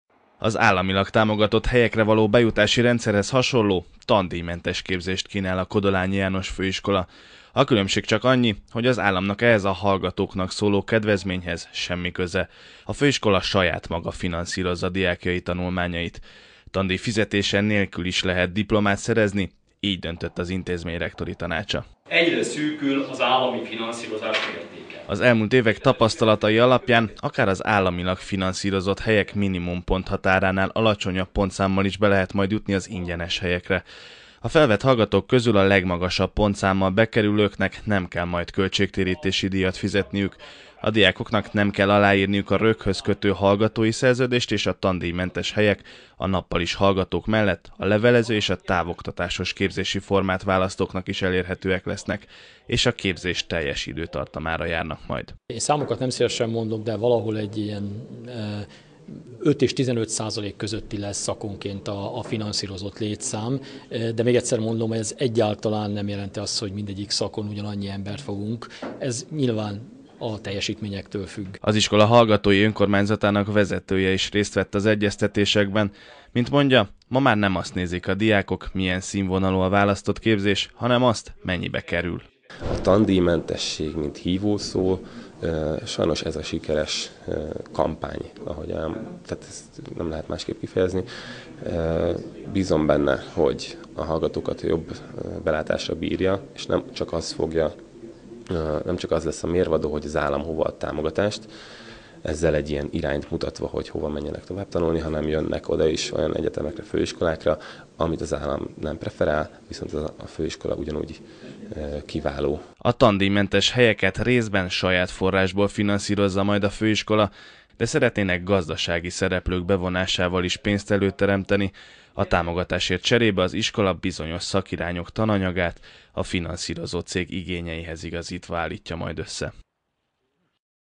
Tandíj nélkül a Kodolányin
Újfajta tandíjmentes képzési formát talált ki a Kodolányi János Főiskola. Az államilag támogatott rendszerhez hasonlóan, ingyenes képzést biztosít leendő hallgatóinak. Az ehhez szükséges összeget azonban nem az állam, hanem az intézmény saját maga biztosítja. A főiskola hallgatóinak 10-15 százaléka a rendszer segítségével tandíj fizetése nélkül szerezheti majd meg diplomáját a jövőben.